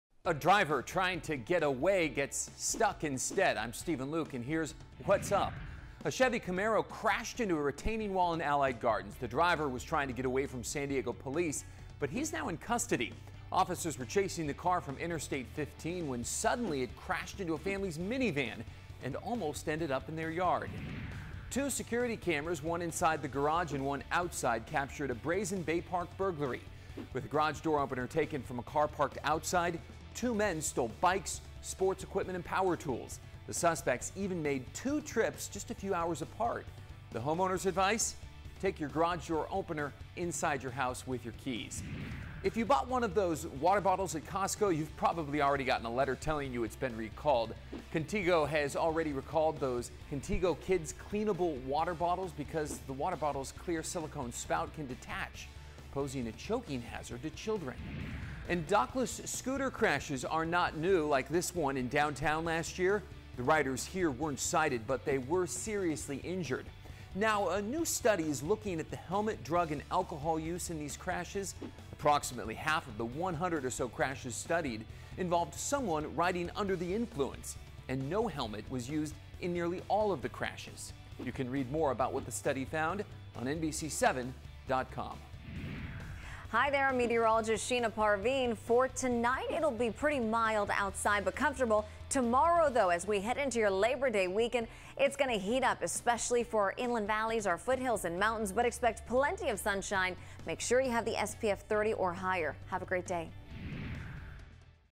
Wall Stops Driver, Brings Police Chase to a Halt | What's Up?
A car crashed into a wall in Allied Gardens, bring a police chase to a halt. NBC 7's Steven Luke has what's up.